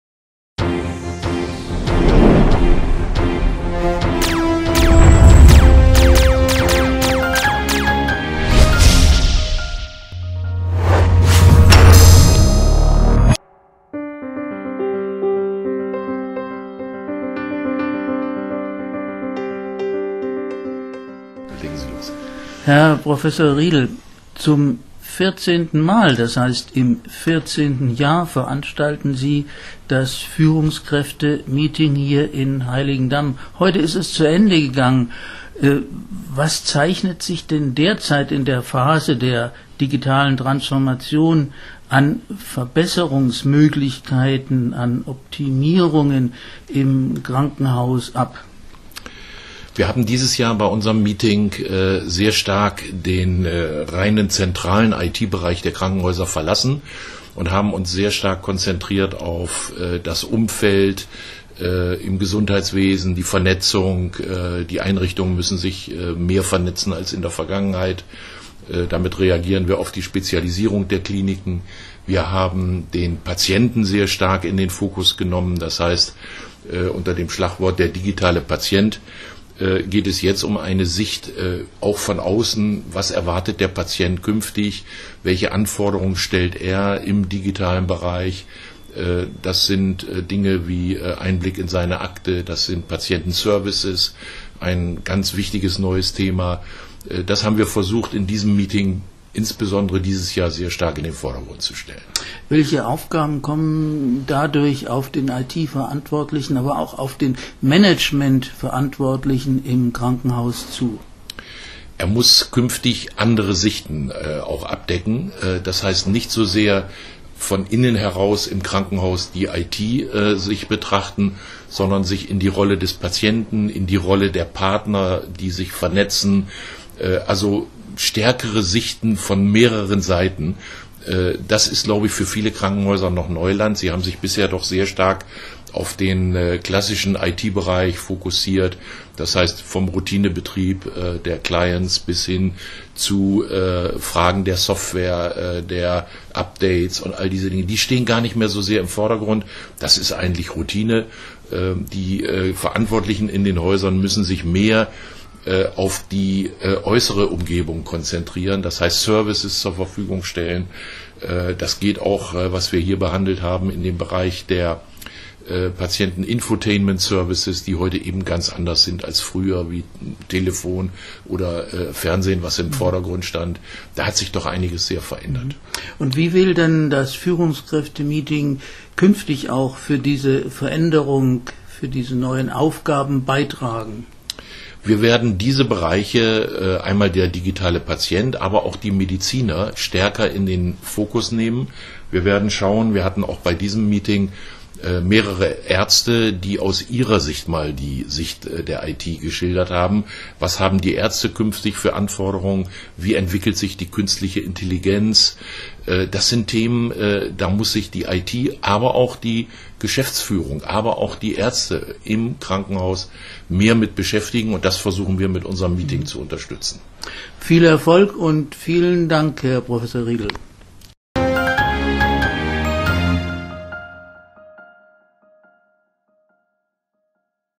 PODCAST: Prof. Dr. Wolfgang Riedel, IfK Braunschweig
Auf dem 14. Meeting-am Meer am 14. und 15. März 2019 in Heiligendamm lautete das Motto „Digitalisierung der Krankenhäuser - der richtige Weg“. Dort sprach Wolf-Dietrich Lorenz, Krankenhaus-IT JOURNAL, mit Gastgeber Prof. Dr. Wolfgang Riedel, IfK Braunschweig über die Themen:

Defizite im Gesundheitswesen in der Phase der digitalen Transformation
Was können Verantwortliche tun?
Wie trägt das „Meeting am Meer“ künftig dazu bei?